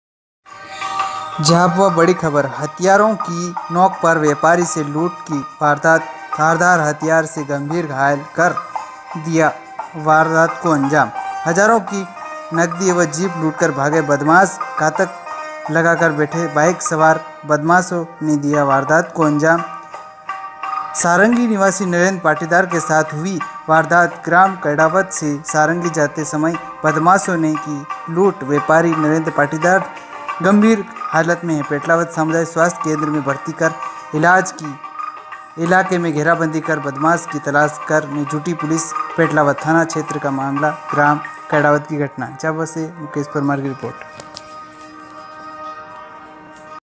झाबुआ बड़ी खबर हथियारों की नोक पर व्यापरी से लूट की वारदात धारदार हथियार से गंभीर घायल कर दिया वारदात
झाबुआ बड़ी खबर-हथियारों की नोक पर व्यापारी से लूट की वारदात, धारदार हथियार से गंभीर घायल कर दिया वारदात को अंजाम, हजारों की नकदी व जीप लूटकर भागे बदमाश, घात लगाकर बैठे बाइक सवार बदमाशों ने दिया वारदात को अंजाम, सारंगी निवासी नरेंद्र पाटीदार के साथ हुई वारदात, ग्राम करवड़ से सारंगी आते समय बदमाशों ने की लूट, व्यापारी नरेंद्र पाटीदार गंभीर हालत में पेटलावद सामुदायिक स्वास्थ्य केंद्र में भर्ती, इलाके की घेराबंदी कर बदमाशों की तलाश में जुटी पुलिस, पेटलावद थाना क्षेत्र के करवड़ ग्राम की घटना। मो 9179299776